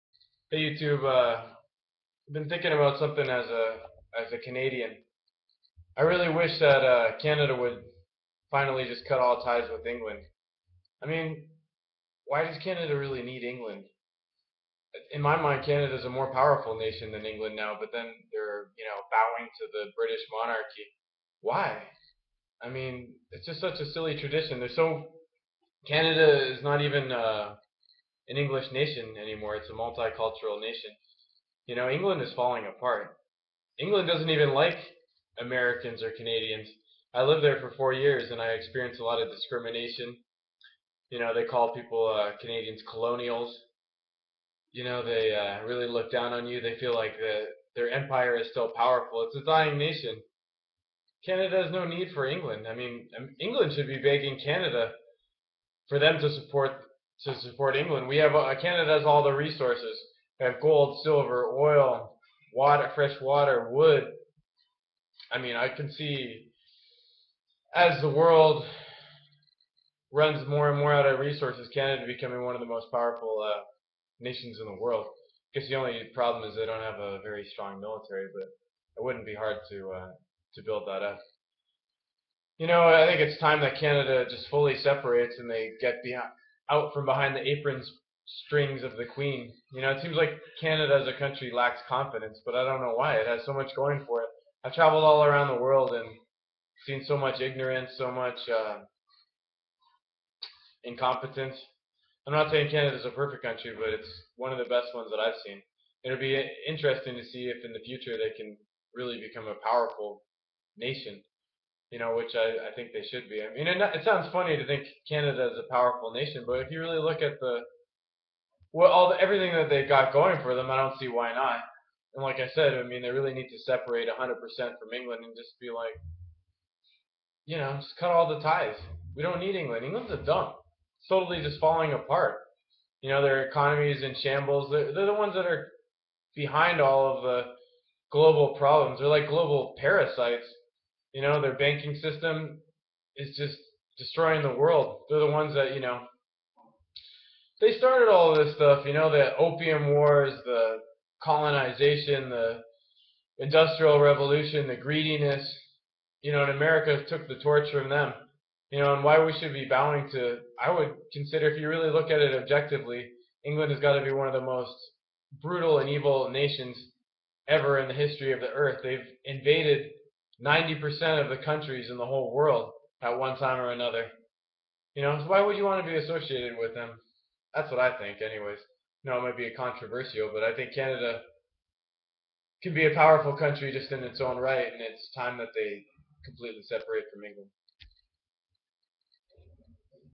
Canada Needs To Bin The Monarchy
Isn't it time for Canada to completely cut ties from England